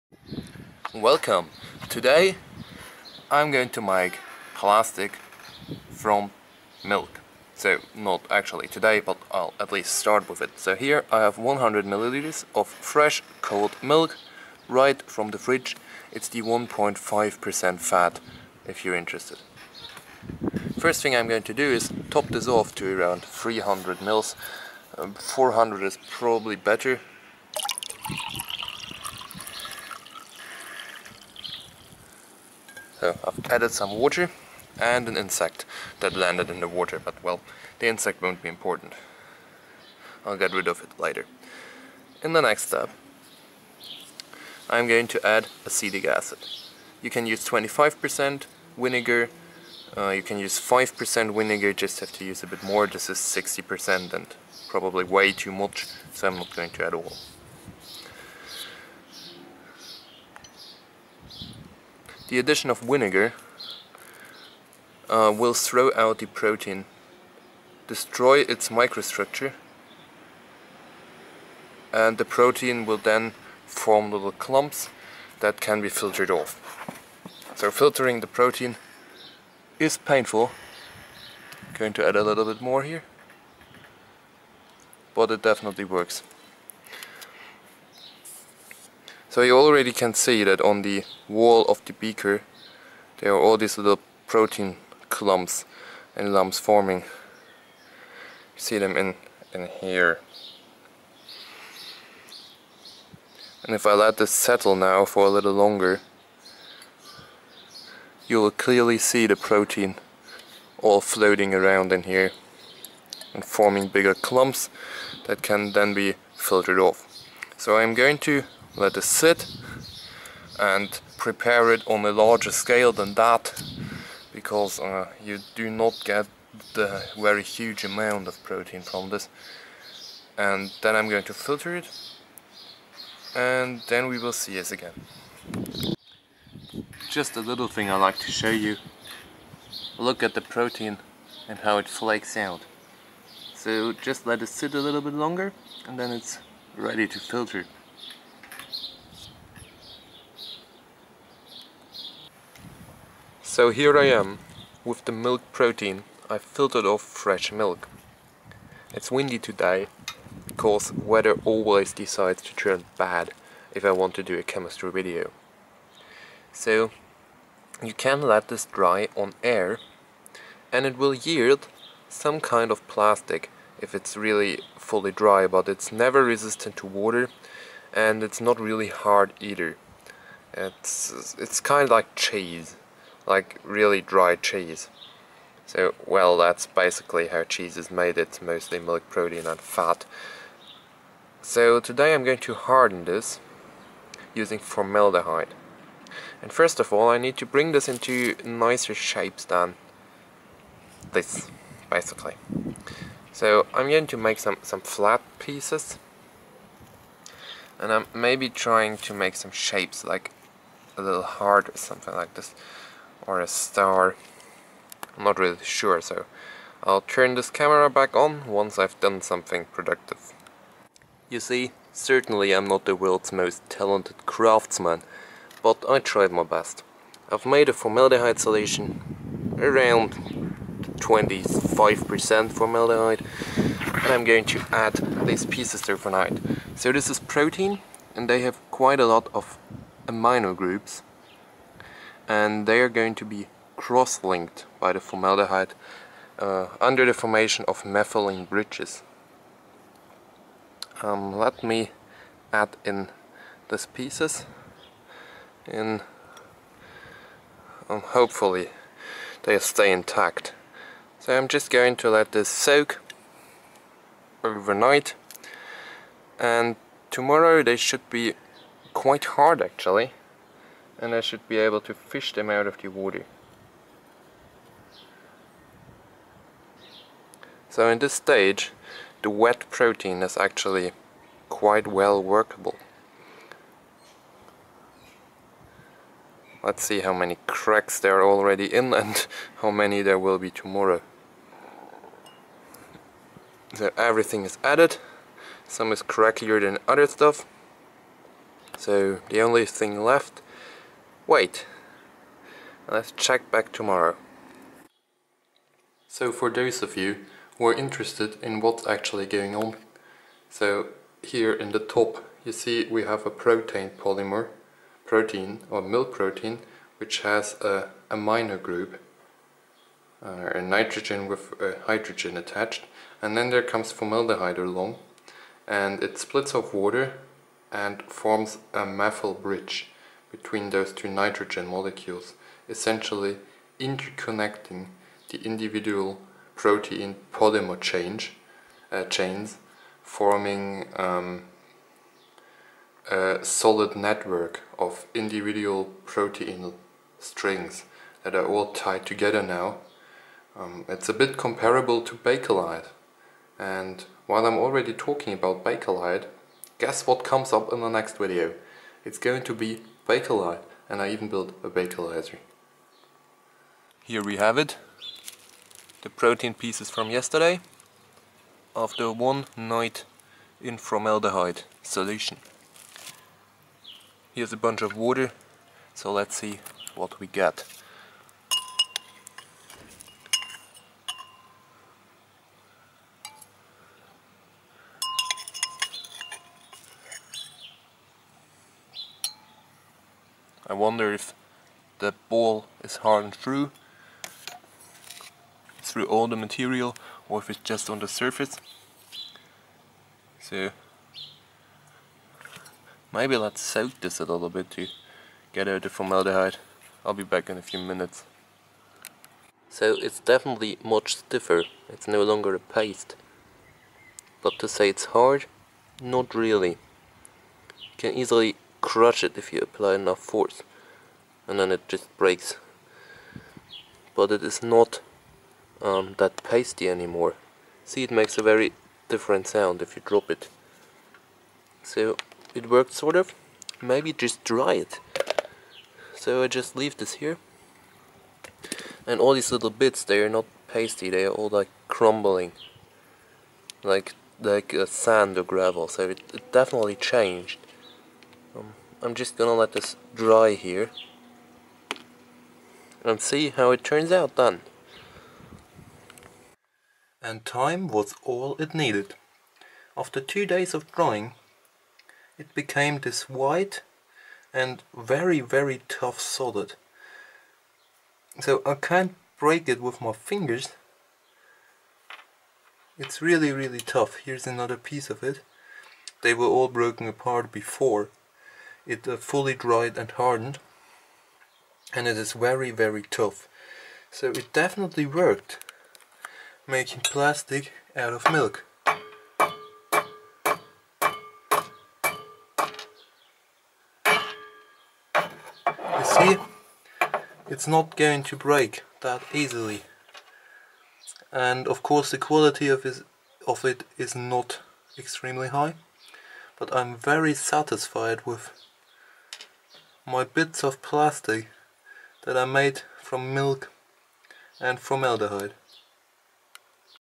Plastic From Milk - Making Galalith
In this video I make plastic from milk. It is no new fancy approach for more green plastics, instead Galalith is one of the oldest plastics made and has gone extinct since then. Time for a revival?